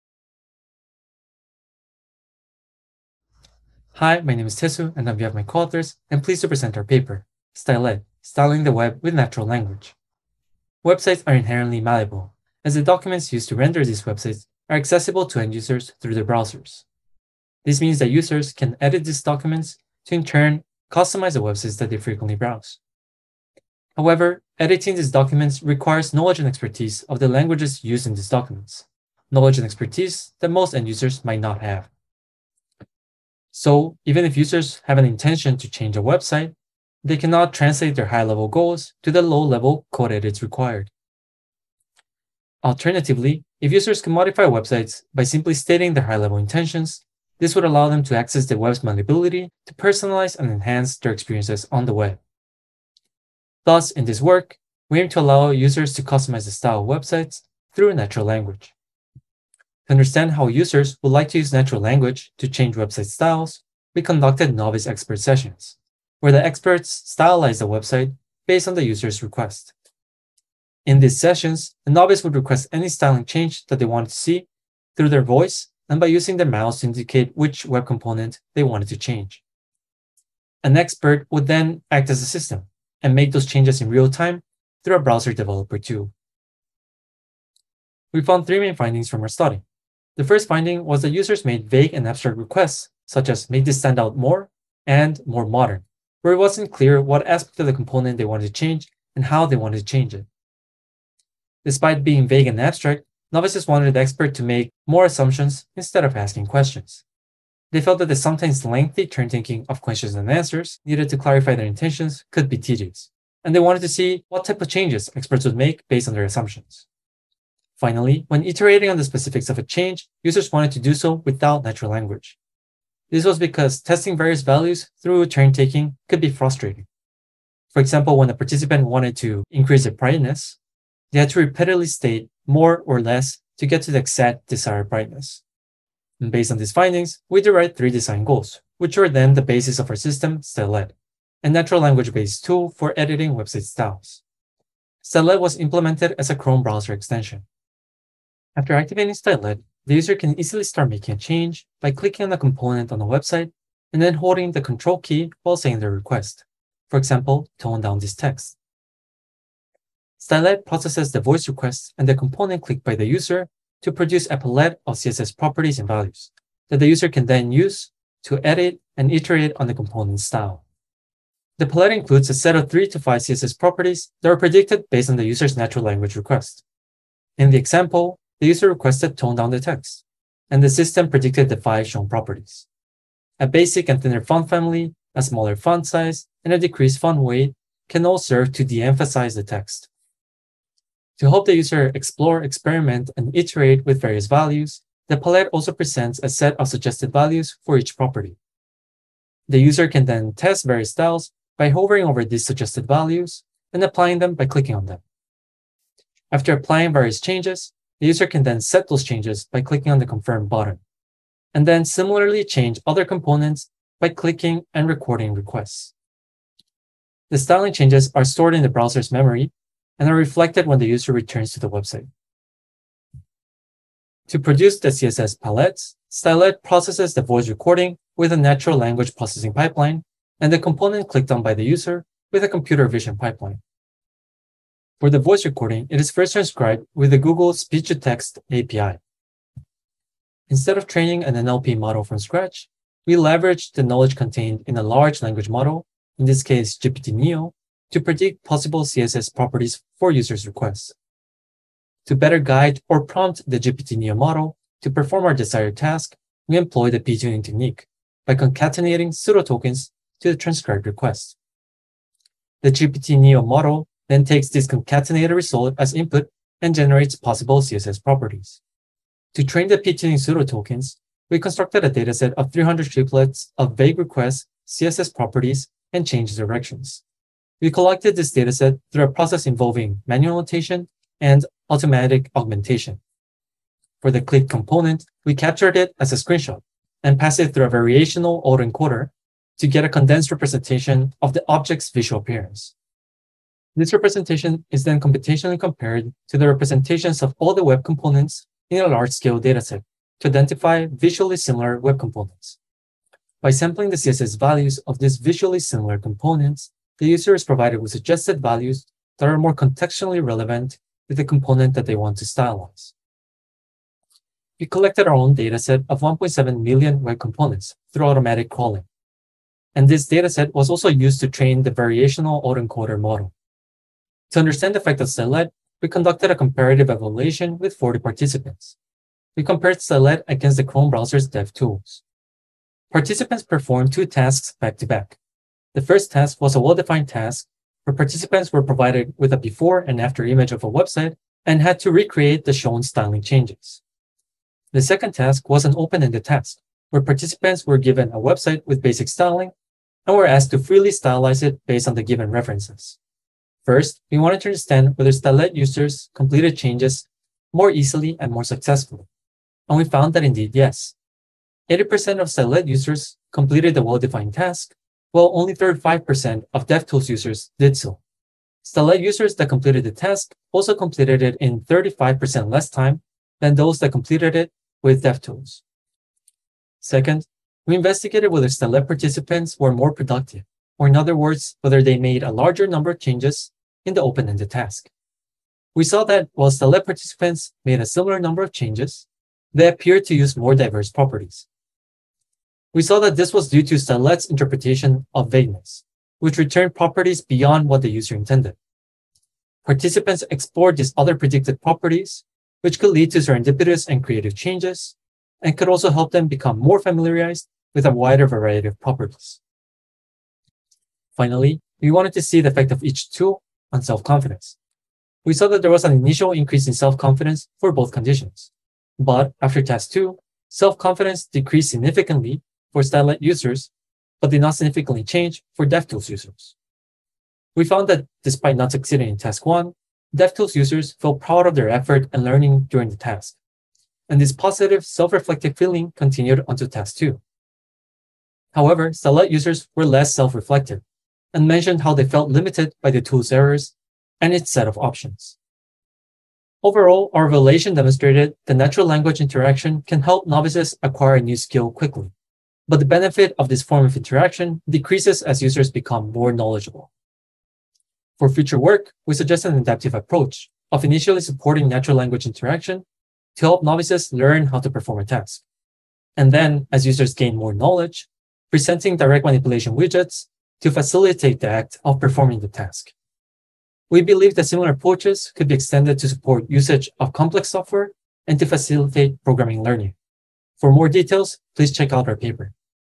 Stylette: Styling the Web with Natural Language
Tae Soo Kim, DaEun Choi, Yoonseo Choi, Juho Kim

Session: AI: Design and studies

Abstract
End-users can potentially style and customize websites by editing them through in-browser developer tools. Unfortunately, end-users lack the knowledge needed to translate high-level styling goals into low-level code edits. We present Stylette, a browser extension that enables users to change the style of websites by expressing goals in natural language. By interpreting the user's goal with a large language model and extracting suggestions from our dataset of 1.7 million web components, Stylette generates a palette of CSS properties and values that the user can apply to reach their goal. A comparative study (N=40) showed that Stylette lowered the learning curve, helping participants perform styling changes 35% faster than those using developer tools. By presenting various alternatives for a single goal, the tool helped participants familiarize themselves with CSS through experimentation. Beyond CSS, our work can be expanded to help novices quickly grasp complex software or programming languages.